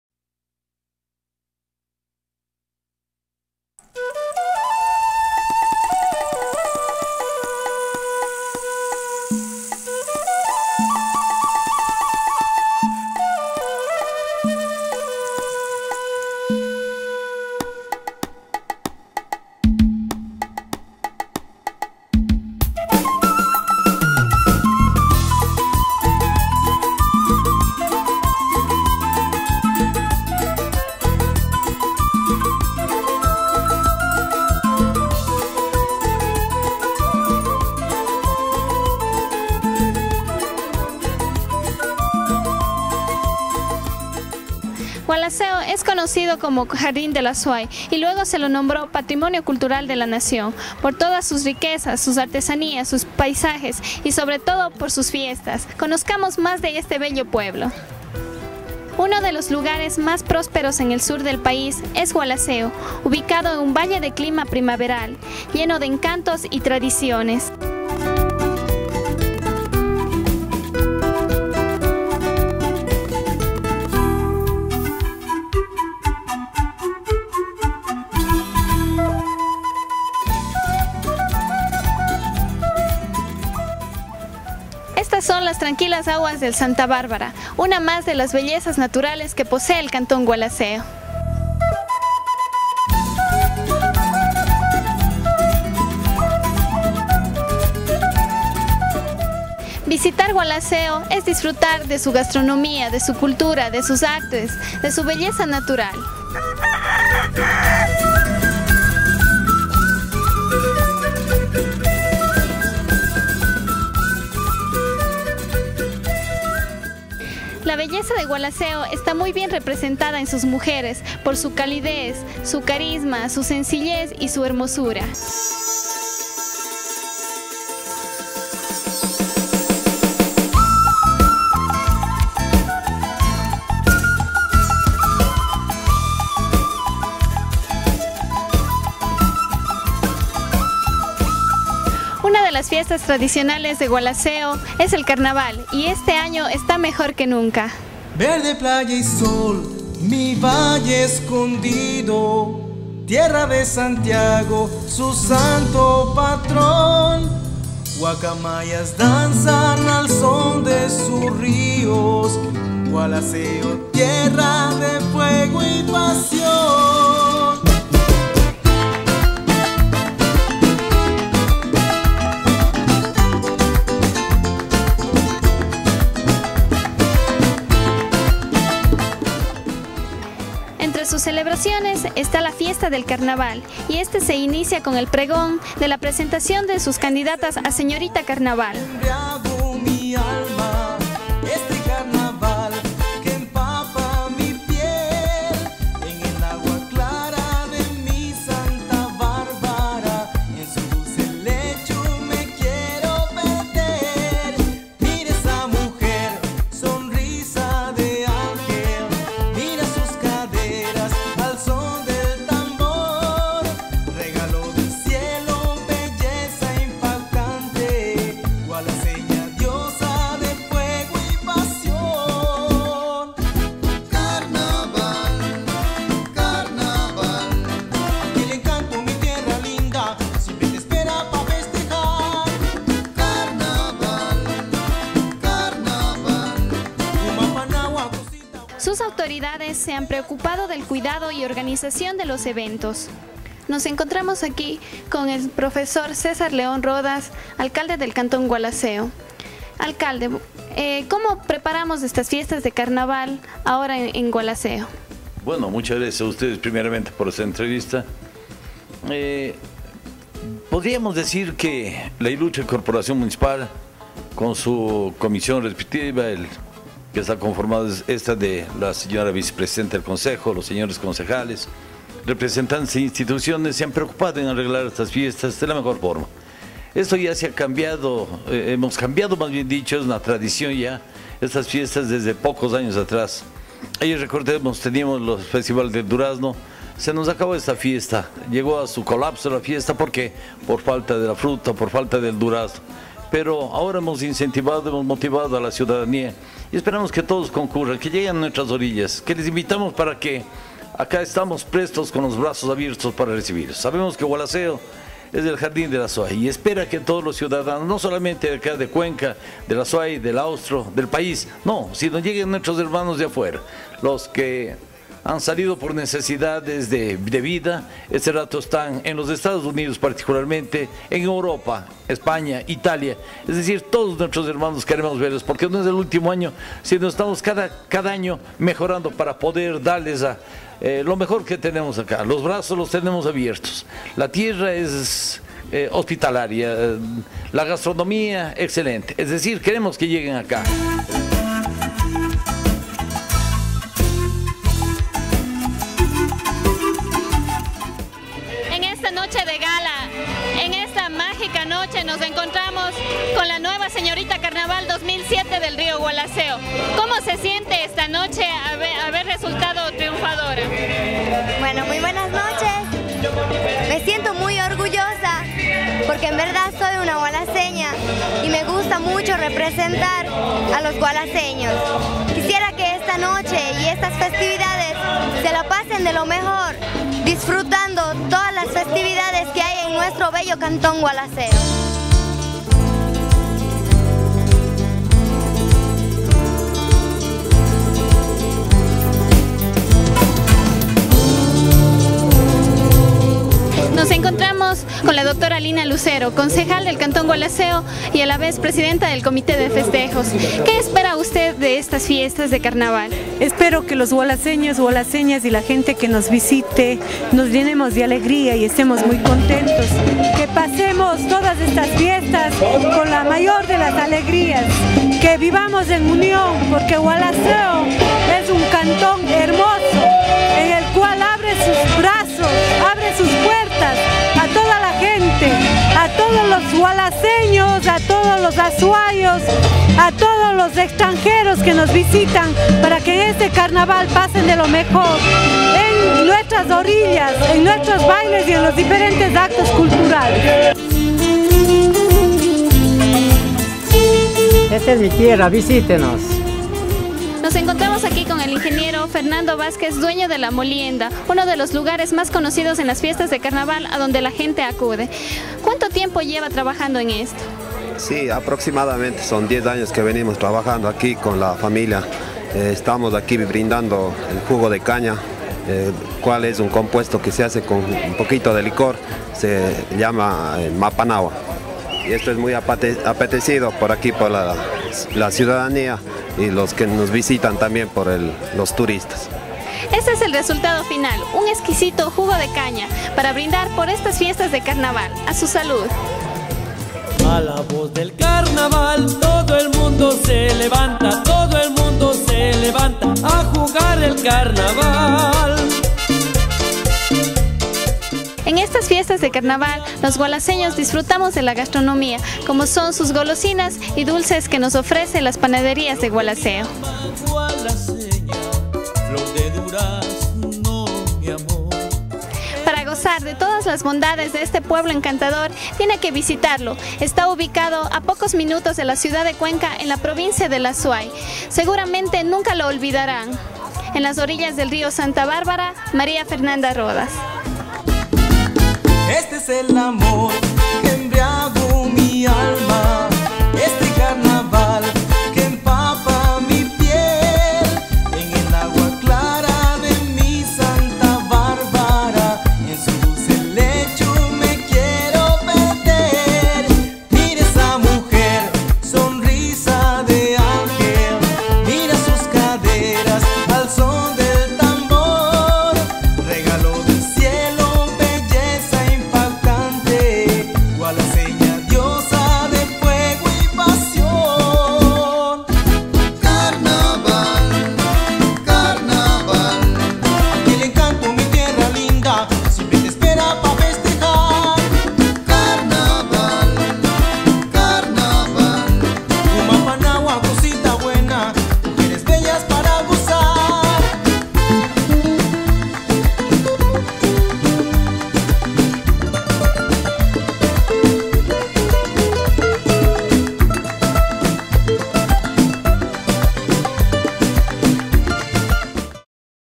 GUALACEO CARNAVAL 2007
DOCUMENTAL DE GUALACEO Y SUS BELLEZAS NATURALES ASI COMO DE SUS FIESTAS DE CARNAVAL, CREADO Y PRODUCIDO POR "VISTAS PRODUCTIONS" NEW YORK-USA. Y CONDUCIDO POR MARIA FERNANDA RODAS, ESTE REPORTAJE FUE TRANSMITIDO POR ECUAVISA INTERNACIONAL. VISITE VISTASPRODUCTIONS.COM PARA MAS DETALLES